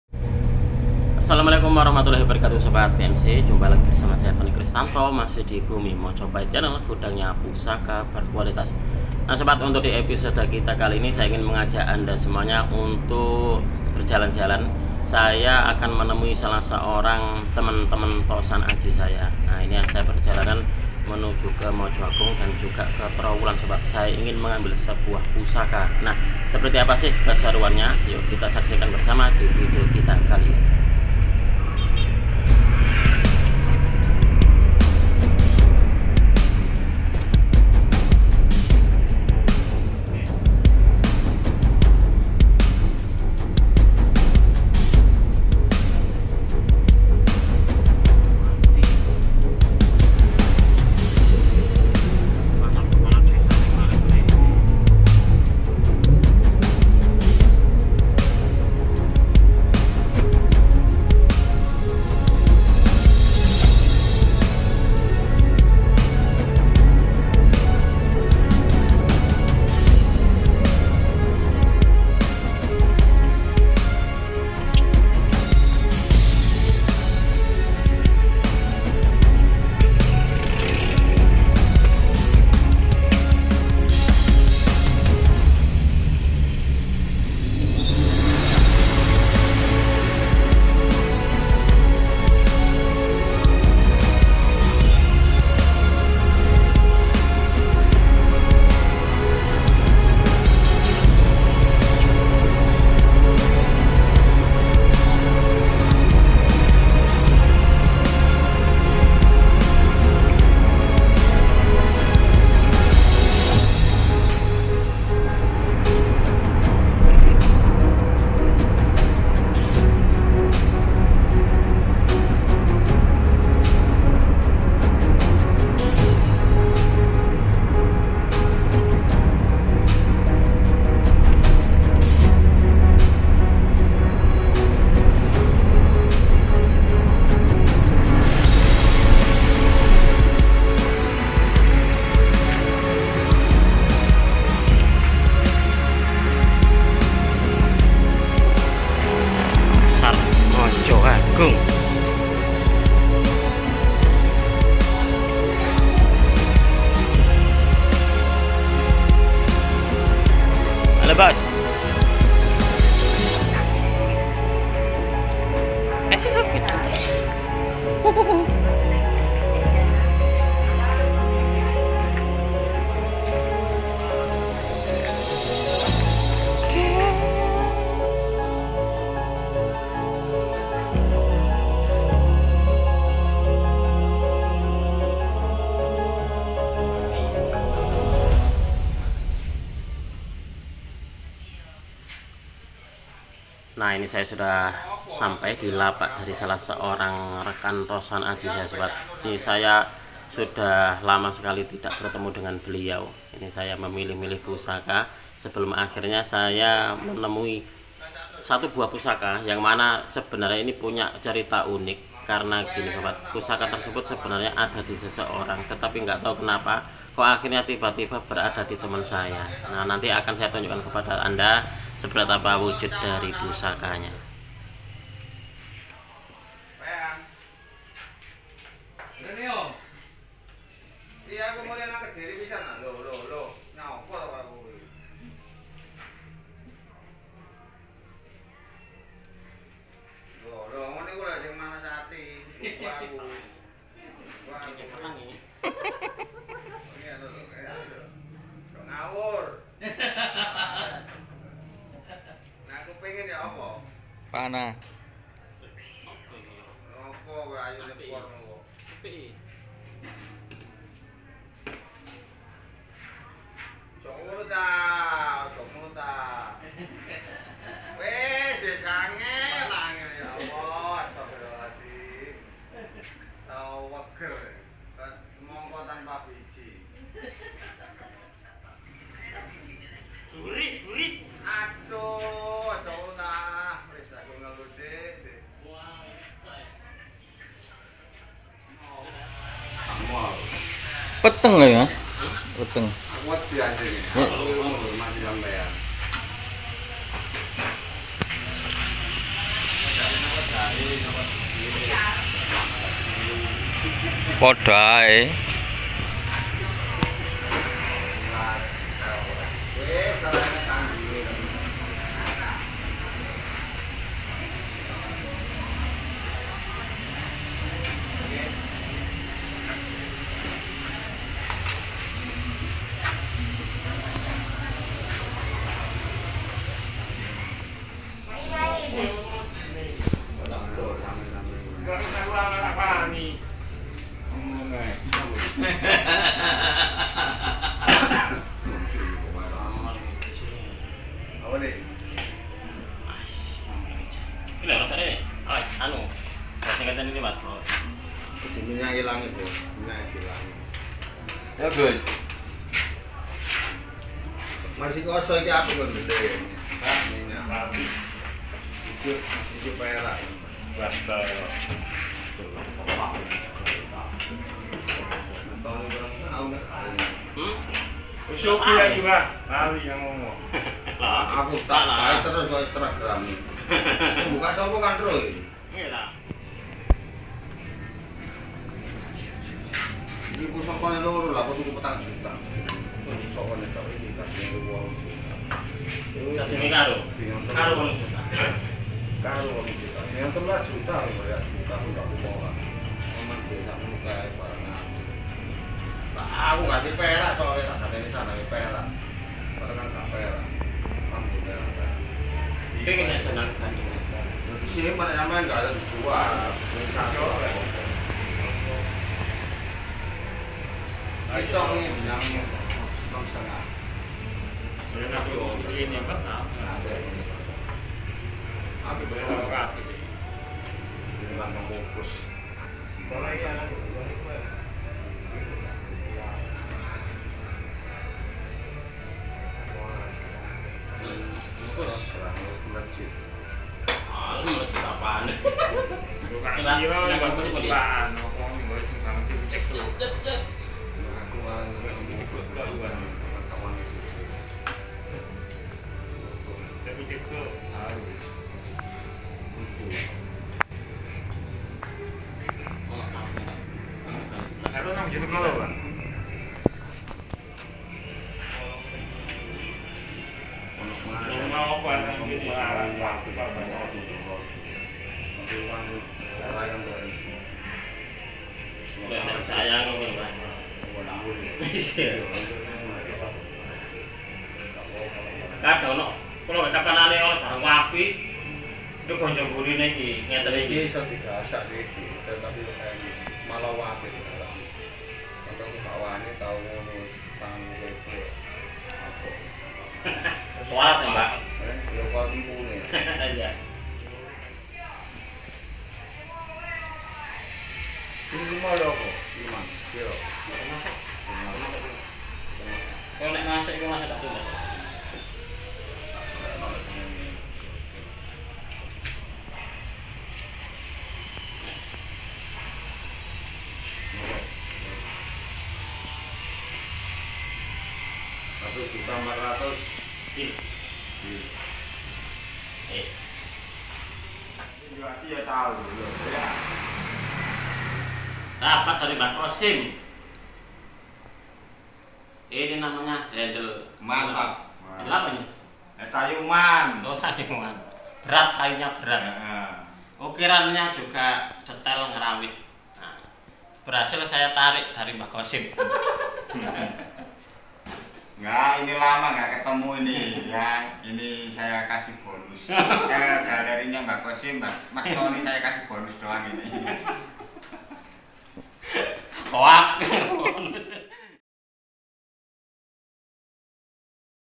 MENJEMPUT KERIS PUSAKA DI MOJOAGUNG
Hallo sobat semua para pecinta tosan aji di Bumi Mojopahit Channel.

Di video kali ini saya akan mengajak kalian semua untuk belajar bersama tentang sebuah tosan aji yang saya beri judul MENJEMPUT KERIS PUSAKA DI MOJOAGUNG

Ini adalah beberapa koleksi pusaka terbaru galeri Bumi Mojopahit.
Dan yang saya review disini adalah keris yang saya peroleh dari seorang teman di Mojoagung.
Nah, seperti apakah pusakanya ? Kita saksikan bersama di video ini.

Semoga video ini bermanfaat bagi kalian semua khususnya bagi para pecinta tosan aji pemula.

Salam dari Bumi Mojopahit
Toni Kristanto